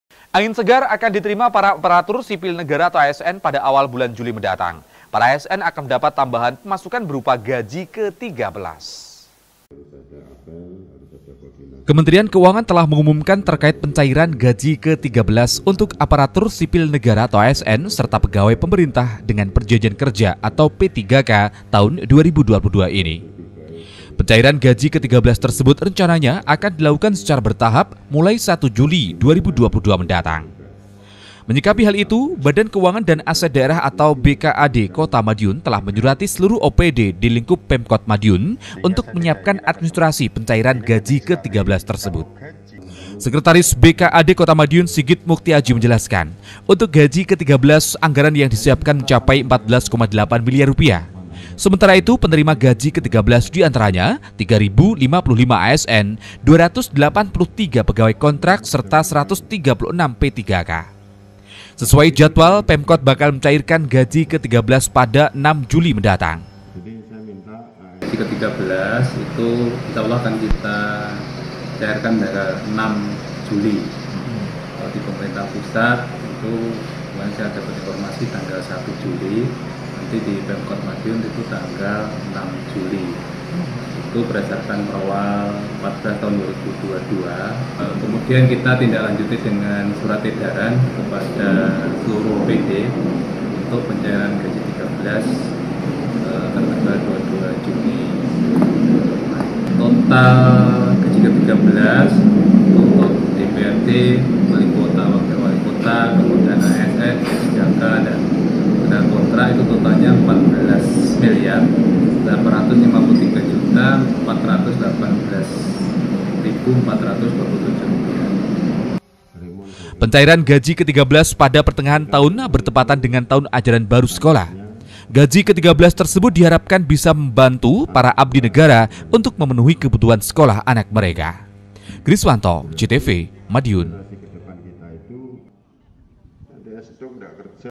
KOTA MADIUN - PEMKOT Siapkan 14.8 Miliar Untuk Gaji Ke-13
Angin segar akan diterima para aparatur sipil negara (asn) pada awal bulan juli mendatang. Para asn akan mendapatkan tambahan pemasukan berupa gaji ke-13. Untuk di lingkup pemerintah kota madiun  gaji ke-13 akan diberikan pada 6 juli mendatang.
Office : jl. Sarimulya no 47 Rejomulyo, Kartoharjo, Kota Madiun